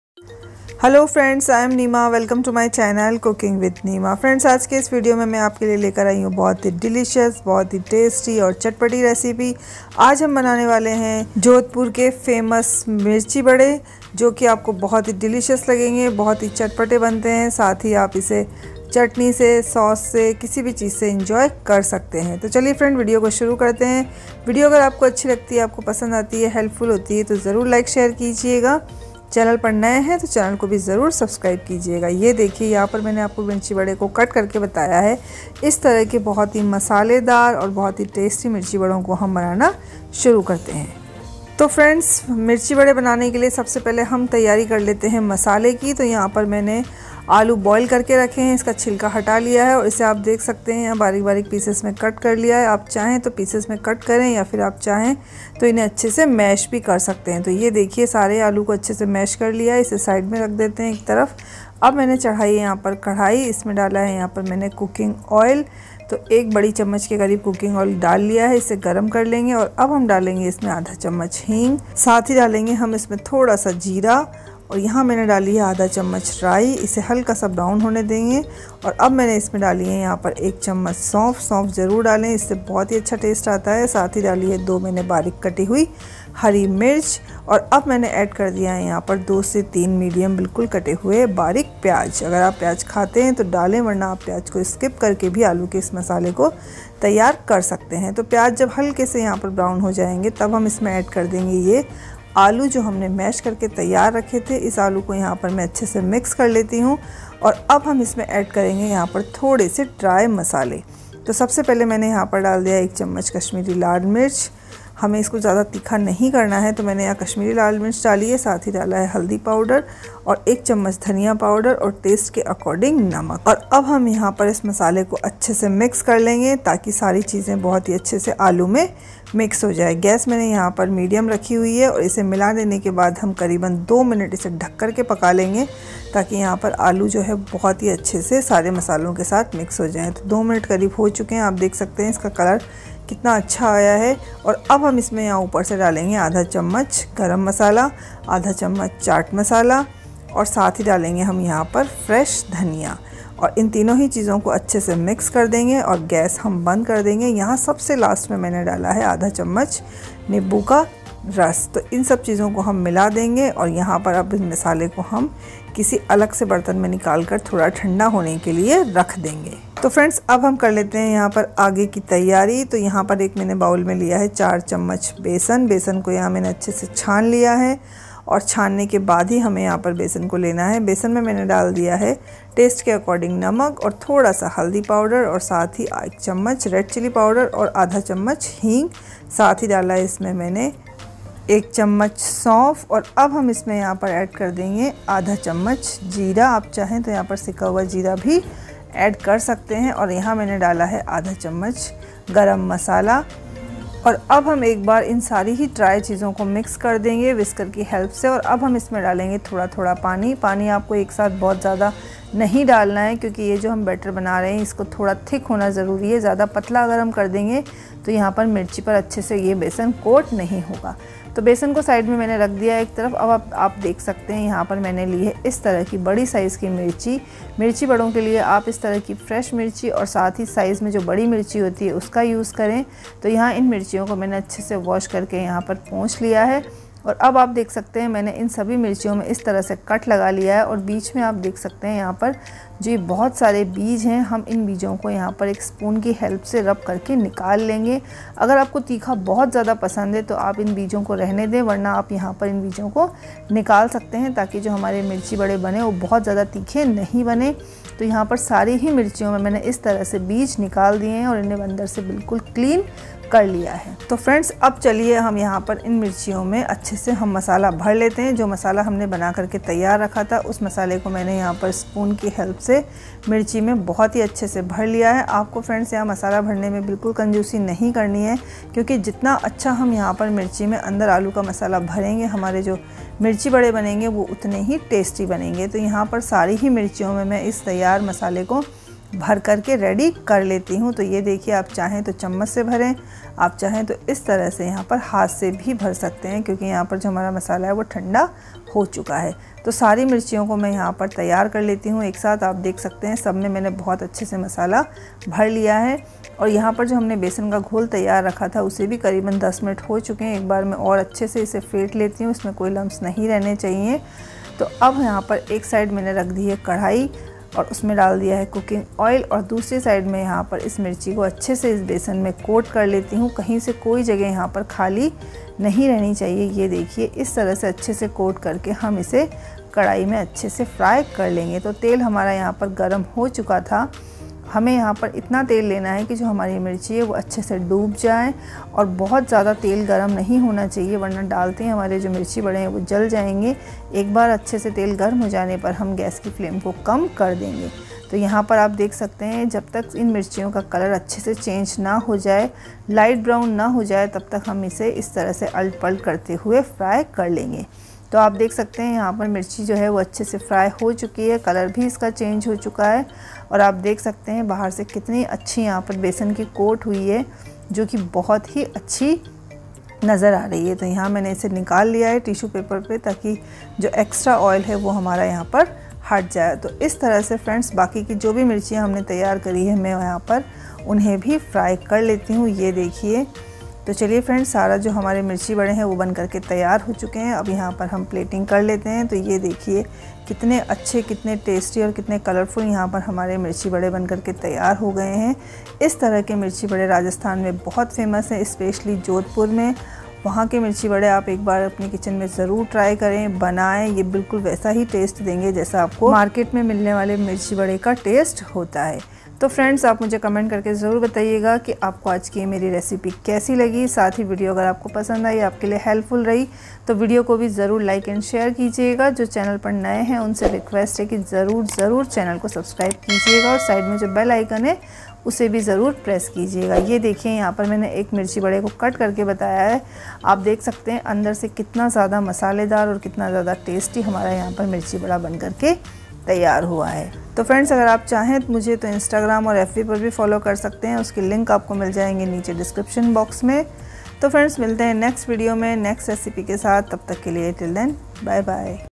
Mirchi vada rajasthani recipe | jodhpuri style mirchi vada | Mirchi vada street style | mirchi bada
Mirchi vada rajasthani recipe | jodhpuri style mirchi vada | mirchi vada street food | mirchi vada kaise banaye

Hello, everyone today in this video I am going to tell u the jodhpur special mirchi vada recipe which is so spicy and delicious also. It can make street style at home.

Jodhpuri Mirchi Vada Recipe :-

• Boiled Potatoes (उबले आलू)
• Cooking Oil (खाने का तेल)
• 1tsp Asafoetida (हींग)
• 1/2 tsp Mustard (राई)
• 1 tsp Cumin (जीरा)
• 1 tsp Fennel (सौंफ)
• 2 Green Chopped Chilli (हरी मिर्च)
• 2-3 Chopped Onion (कटा प्याज)
• 1 tsp Red Chilli Powder (लाल मिर्च पाउडर)
• 1 tsp Turmeric Powder (हल्दी पाउडर)
• 1 tsp Coriandar Powder (धनिया पाउडर)
• 1 tsp Salt (नमक)
• 1 tsp Garam Masala (गरम मसाला)
• 1/2 tsp Chaat Masala (चाट मसाला)
• Coriandar leaves (धनिया पत्ती)
• 1/2 tsp Lemon Water (नींबू पानी)
• 4 tsp Gram Floor (बेसन)
• 1/2 tsp Turmeric Powder (हल्दी पाउडर)
• Big Size Chilli (बड़ी मिर्च)